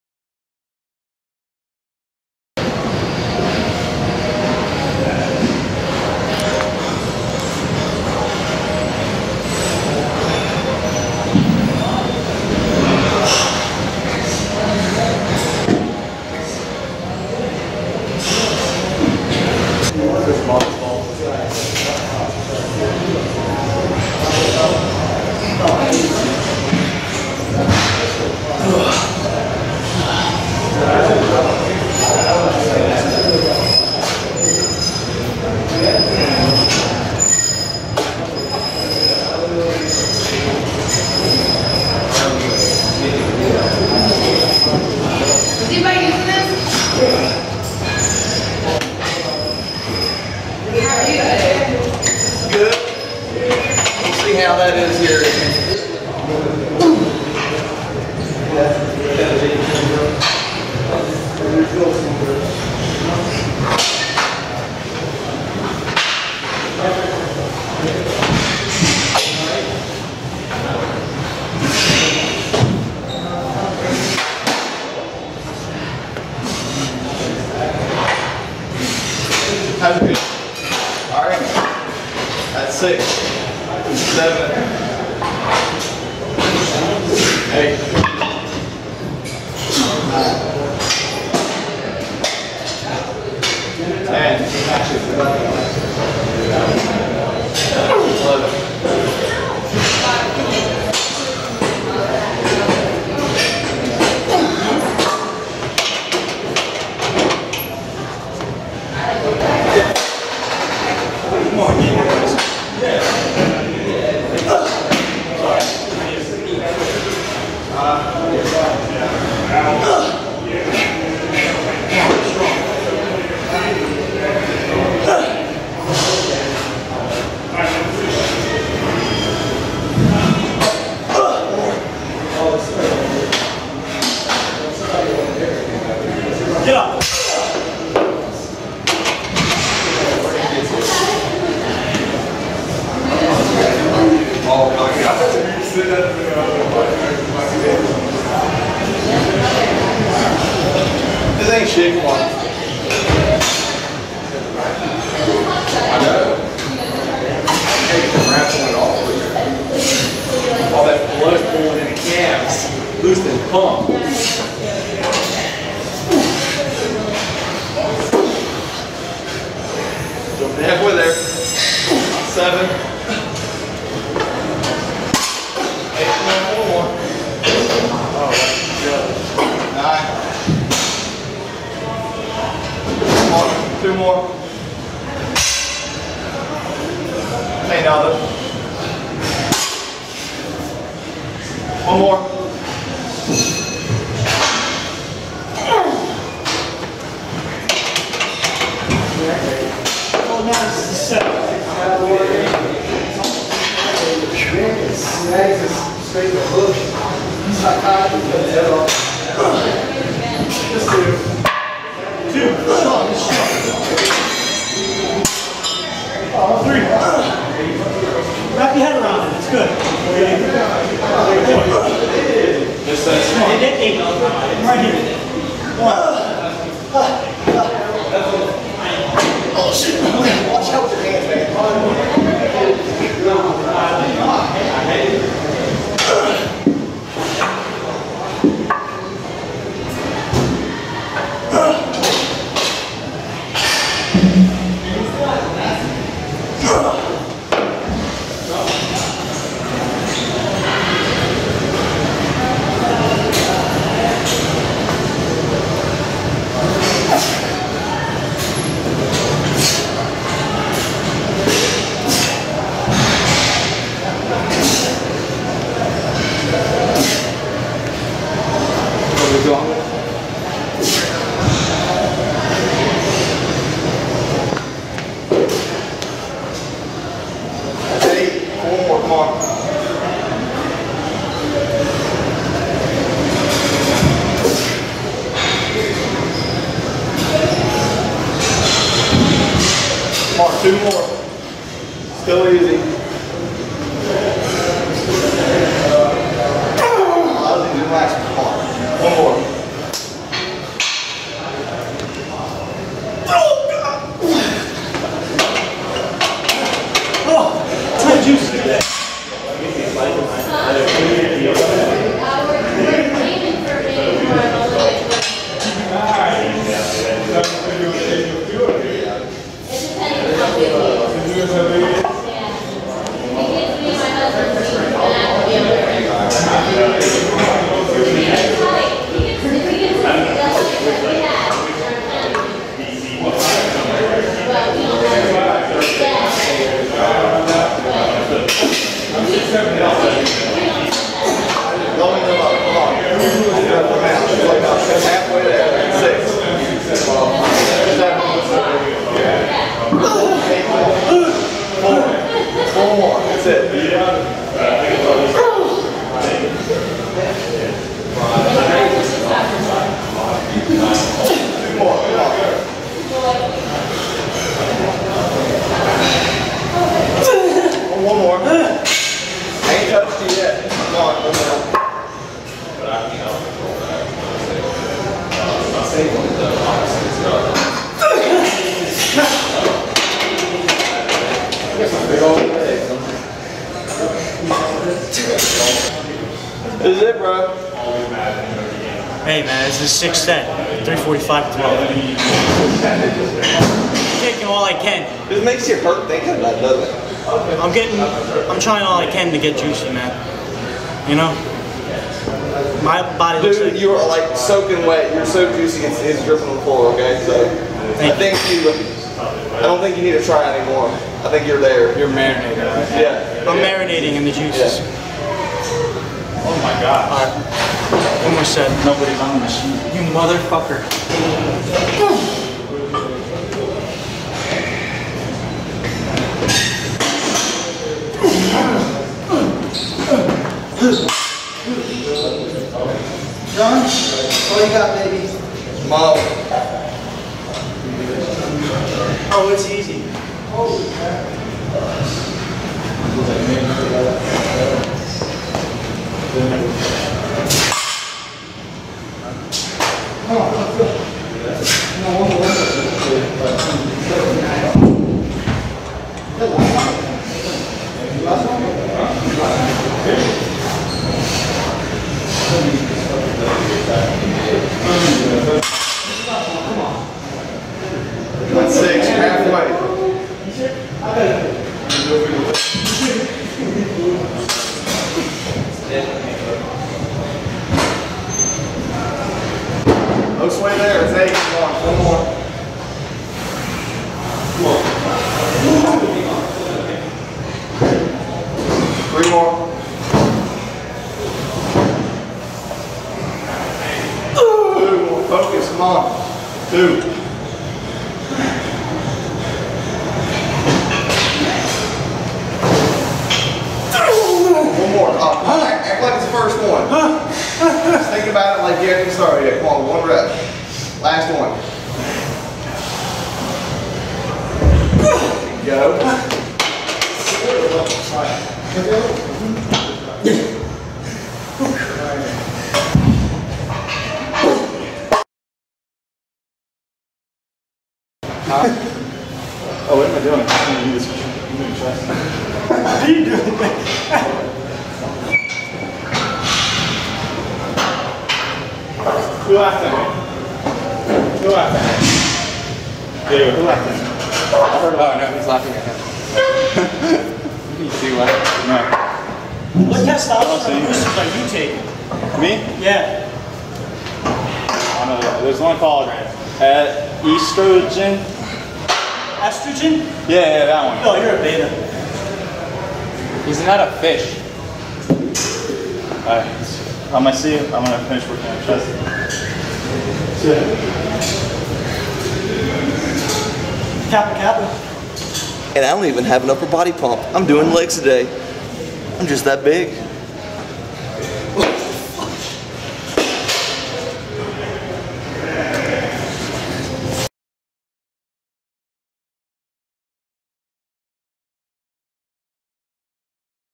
Natural Teen Bodybuilder Squatting Crazy Reps! JUICY!
Squat training in the gym with my training partner and good friend. Try to go in deep with good form and heavy ass weights.